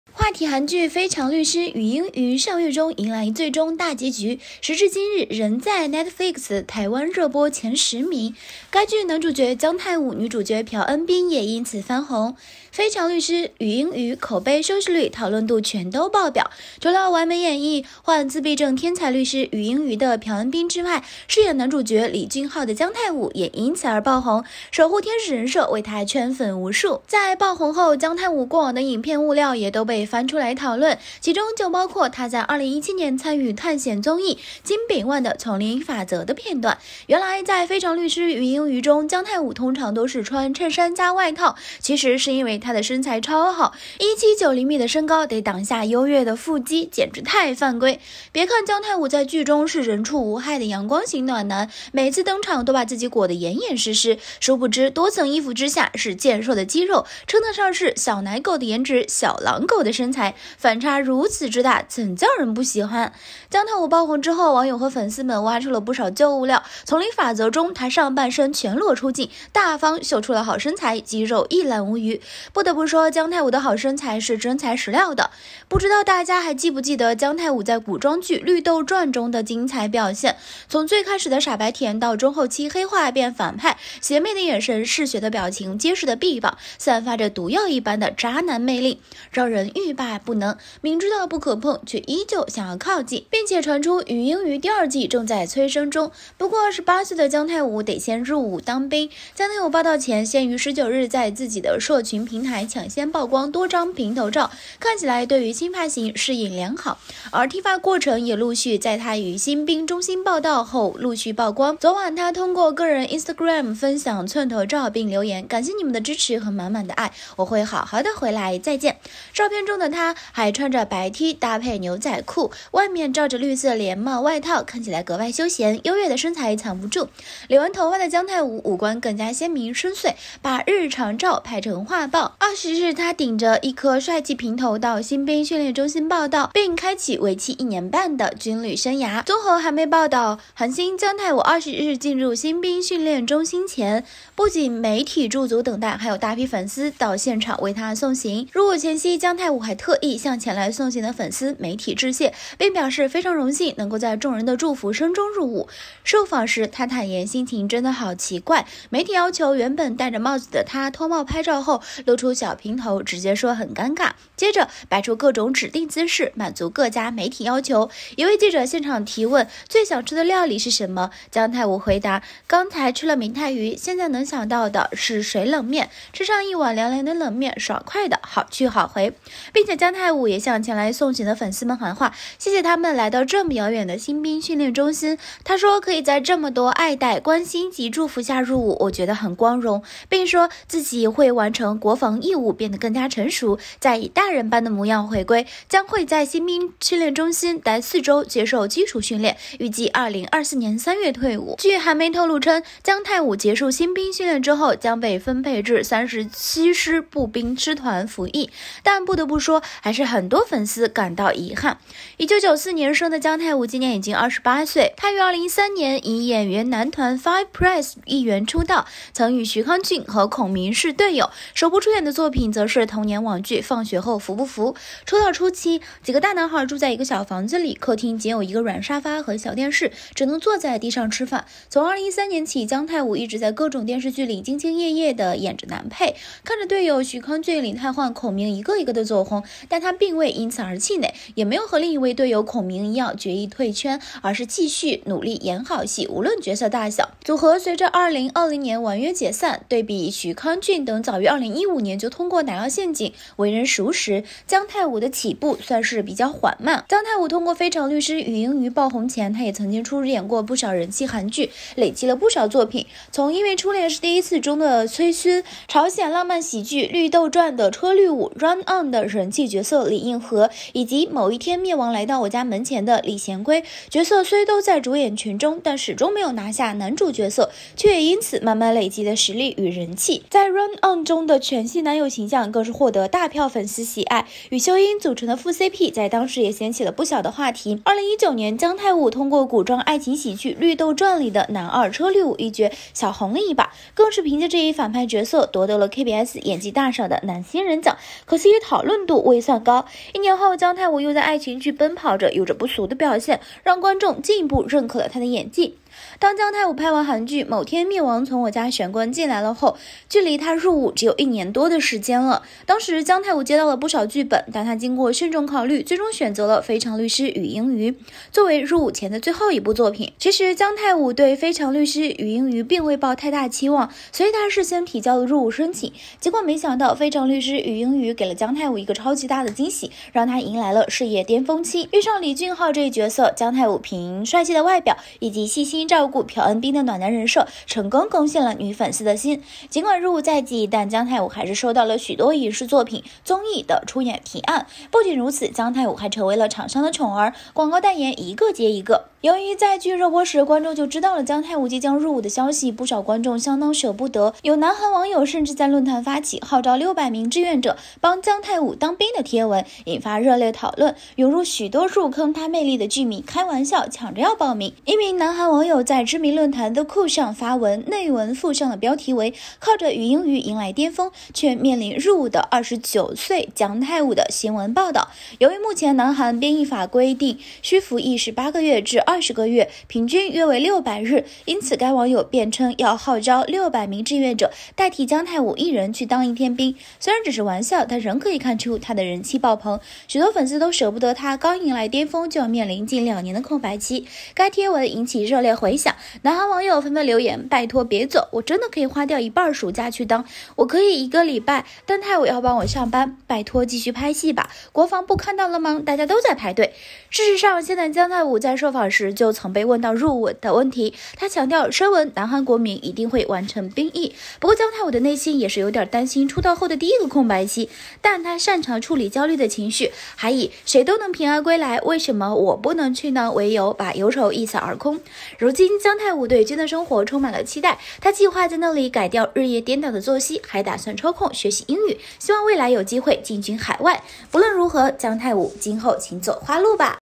姜泰伍入伍現場淚崩，記者採訪神秘女露面，一句話戳中淚點集體炸鍋，禹英隅第二季真的藏不住#姜泰伍#非常律師禹英隅
話題韓劇《非常律師禹英禑》上月中迎來最終大結局
時至今日仍在Netflix台灣熱播前10名
該劇男主角姜泰伍、女主角朴恩斌也因此翻紅
《非常律师禹英禑》口碑、收视率、讨论度全都爆表
除了完美演绎患自闭症天才律师「禹英禑」的朴恩斌之外
饰演男主角「李浚浩」的姜泰伍也因此而爆红
守护天使人设为他圈粉无数

姜泰伍入伍現場淚崩，記者採訪神秘女露面，一句話戳中淚點集體炸鍋，禹英隅第二季真的藏不住